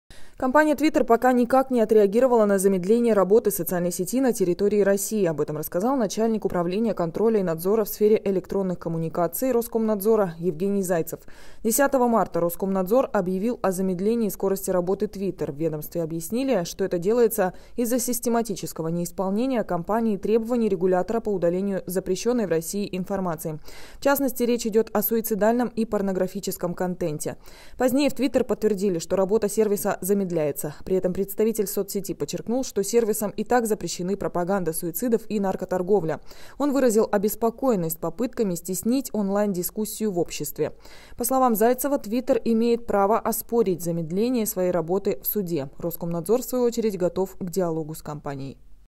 Роскомнадзор пока не дождался реакции Twitter на замедление трафика
Об этом рассказал начальник управления контроля и надзора в сфере электронных коммуникаций Роскомнадзора Евгений Зайцев, сообщает «Интерфакс.
Напомним, 10 марта Роскомнадзор объявил о замедлении скорости работы Twitter. В ведомстве объяснили, что это делается из-за систематического неисполнения компанией требований регулятора по удалению запрещенной в России информации. В частности, речь идет о суицидальном и порнографическом контенте. Позднее в Twitter подтвердили, что работа сервиса замедляется. Он выразил обеспокоенность попытками стеснить онлайн-дискуссию в обществе.
По словам Зайцева, Twitter имеет право оспорить замедление своей работы в суде. Роскомнадзор, в свою очередь, готов к диалогу с компанией.
«Наши претензии остаются без ответа, никто на нас не выходил, на наши вопросы ответов нет. Мы готовы к любому диалогу, лишь бы он был», – передает «Интерфакс» слова представителя регулятора.